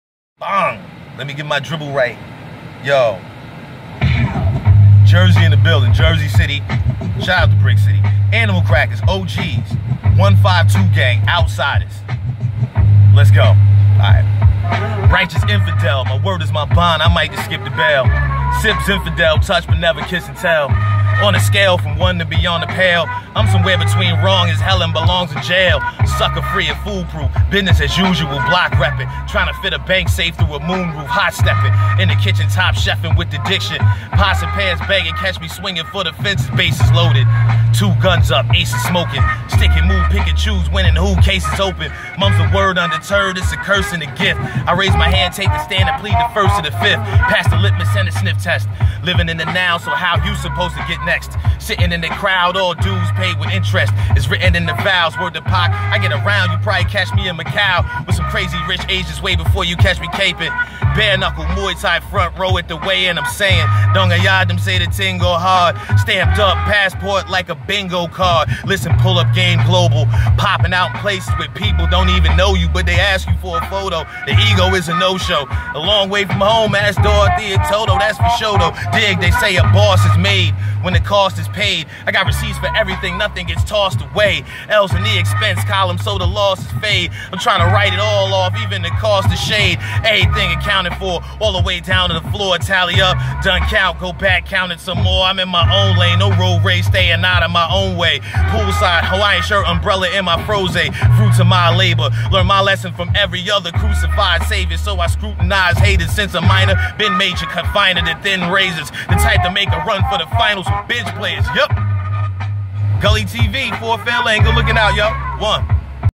Cellphone Flows EPISODE 2 FRD Fairlane- New Jersey
New Jersey MC Ford Fairlane Gives Us A a Display of His Word play wizardry for Cellphone Flows Vol. 2. IF you think you got what it takes to Rock on Gully TV send Us Your HD recorded Cell Phone Freestyle to